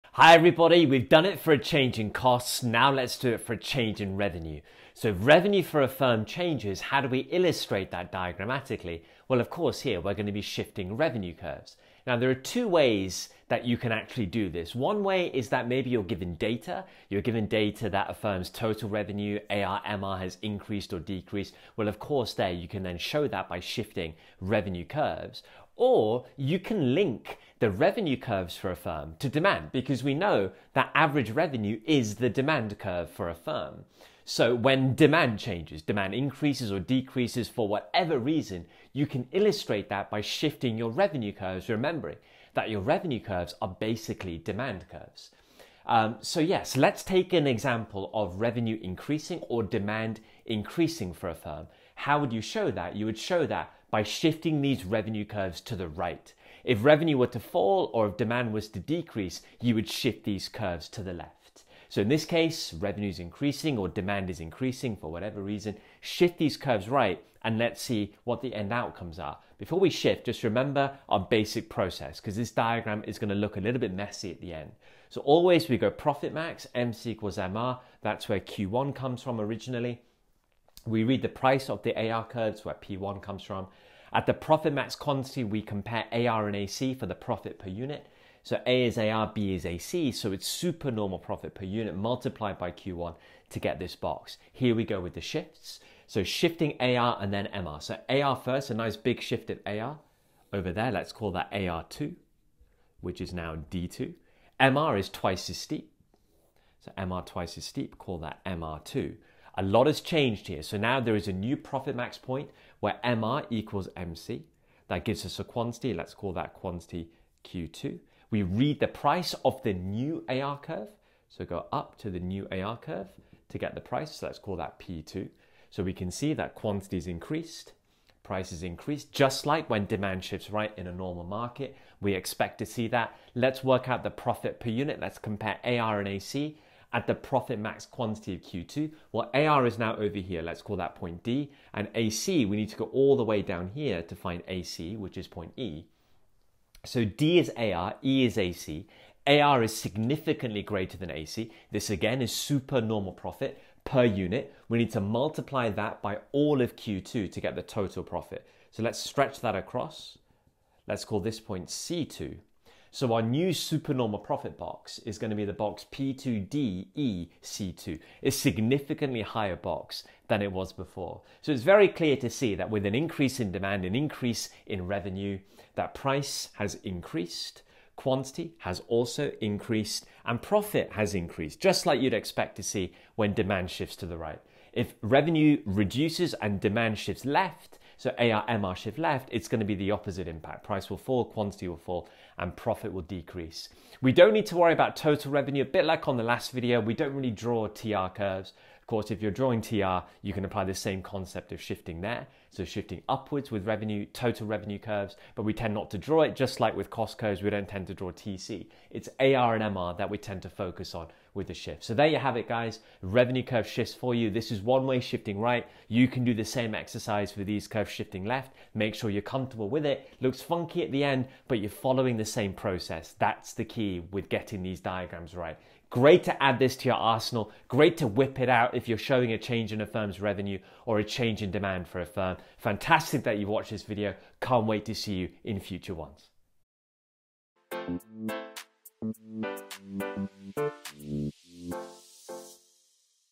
Y2 14) Shifting Revenue Curves - AR & MR
Shifting Revenue Curves - AR & MR. Video covering how to shift revenue curves (AR and MR), perfect Paper 1 revision